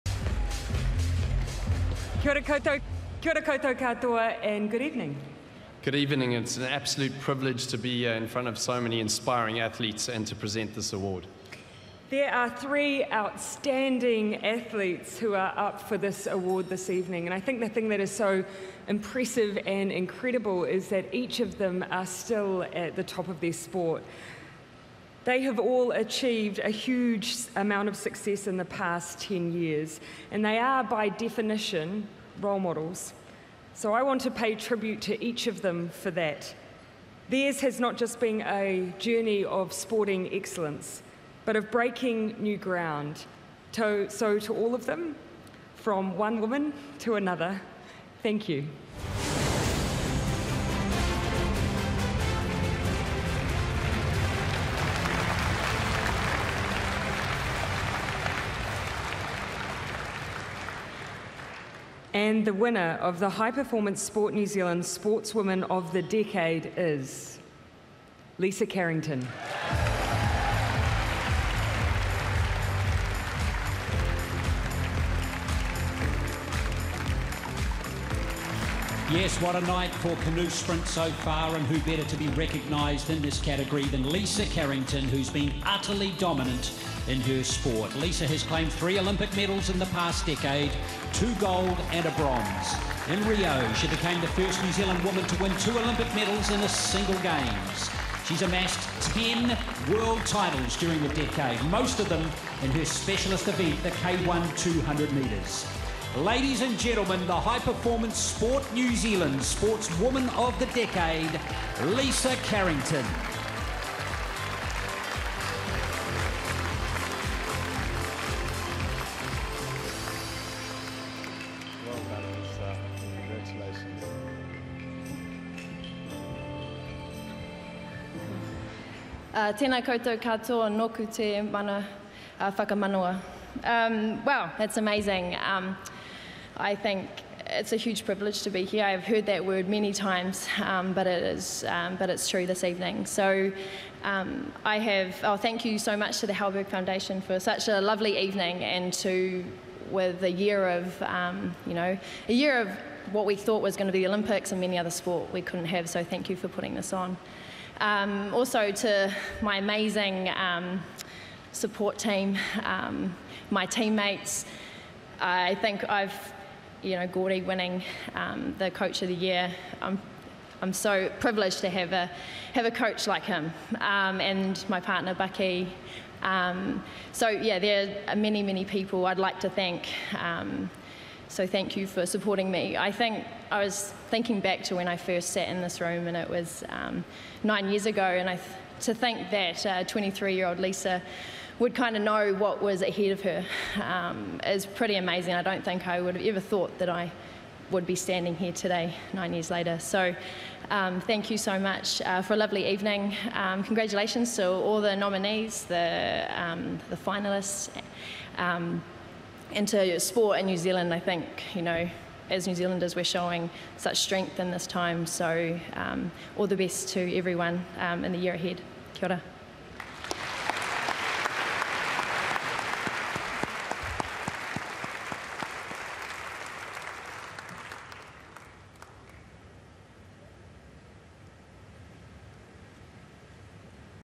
ISPS Handa Halberg Awards Decade Champion - HPSNZ Sportswoman of the Decade Presentation
Watch the ISPS Handa Halberg Awards Decade Champion High Performance Sport New Zealand Sportswoman of the Decade Presentation